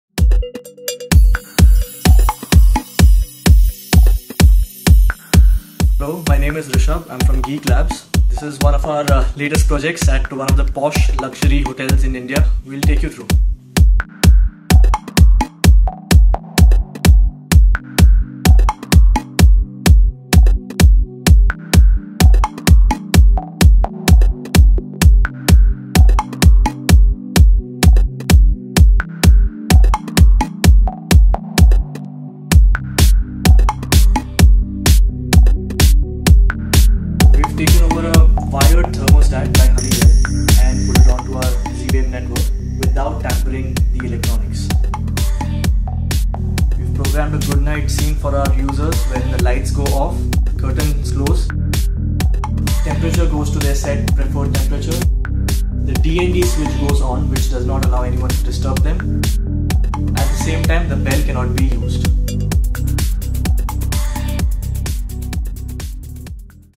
JW Marriot Sahar - Smart Hotel Room - Hotel Automation
Here is a glimpse of our work at the new JW Mariott hotel located next to the Sahar Airport, Mumbai. All of the automation work was done after the interiors of the hotel was completed.